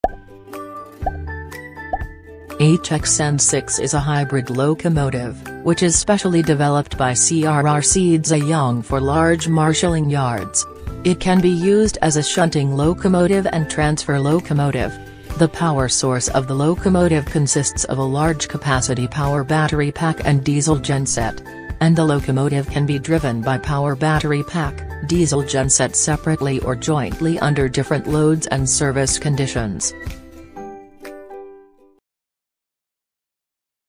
CRRC ZIYANG-HXN6 INTRODUCTION
HXN6 is a hybrid locomotive, which is specially developed by CRRC Ziyang for large marshaling yards. It can be used as a shunting locomotive and transfer locomotive. The power source of the locomotive consists of a large-capacity power battery pack and diesel genset. And the locomotive can be driven by power battery pack, diesel genset separately or jointly under different loads and service conditions.